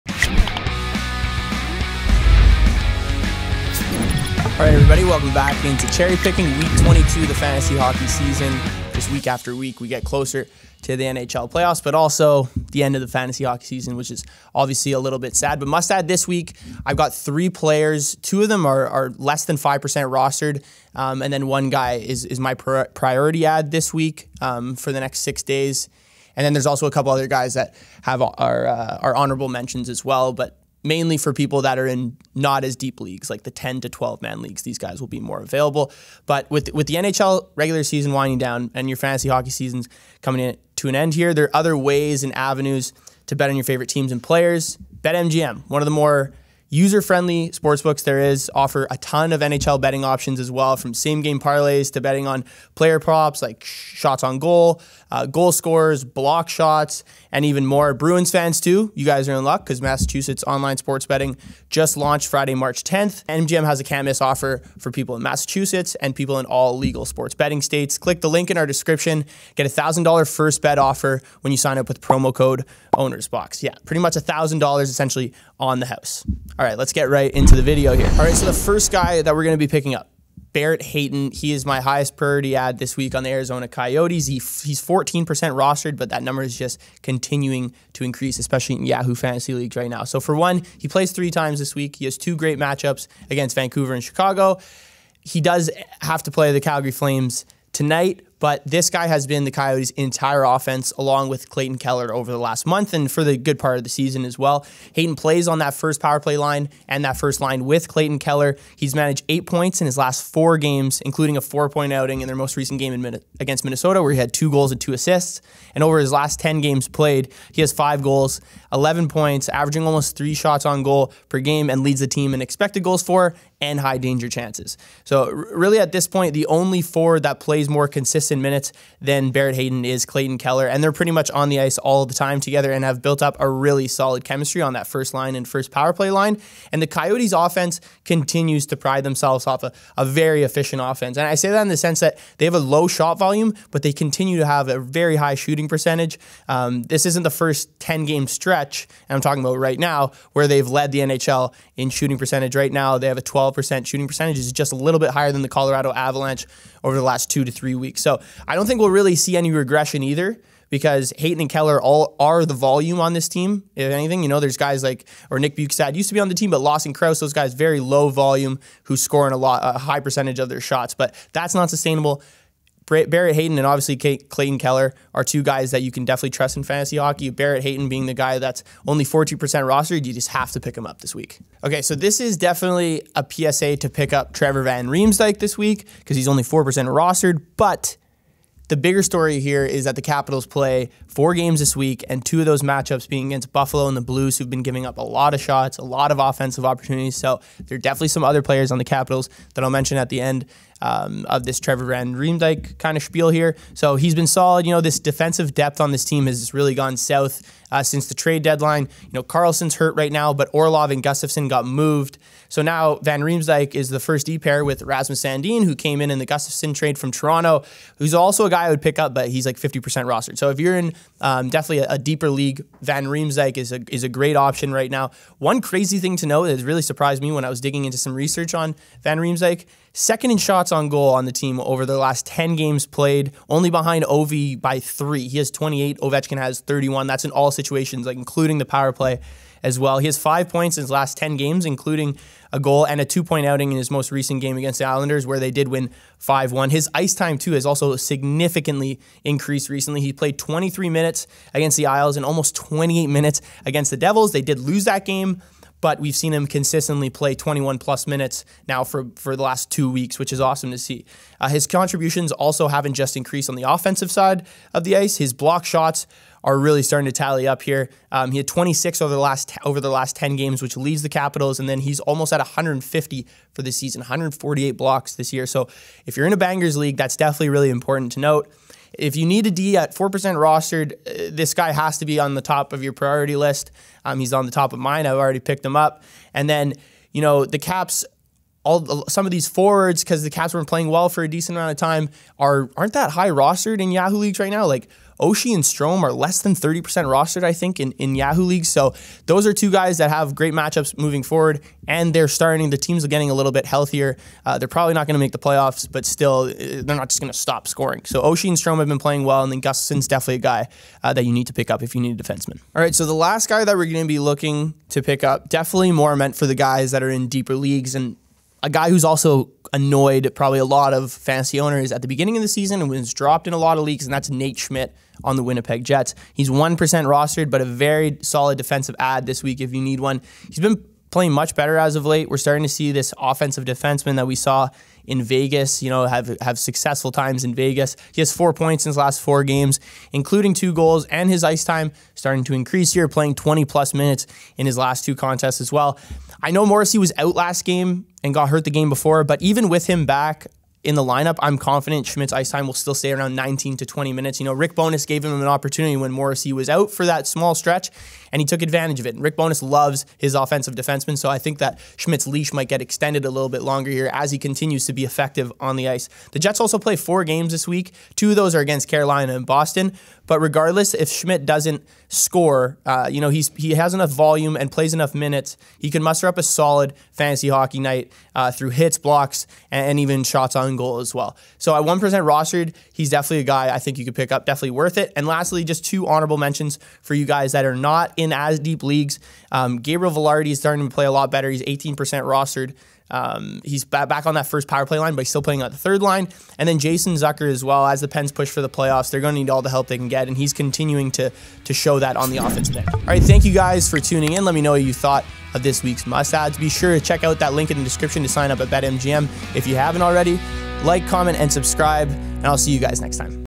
Must Add Players | Week 22 | Fantasy Hockey 2023 | Cherry Pickin' Podcast
Cherry Pickin' host Ryan Sura breaks down the best Fantasy Hockey players to add for Week 22. Defensemen are on the mind with two MUST ADDS on the backend in Fantasy Hockey this week.

1:22 - Barrett Hayton (Coyotes)
3:02 - Trevor van Riemsdyk (Capitals)
5:24 - Nate Schmidt (Jets)
BETMGM SIGN UP OFFER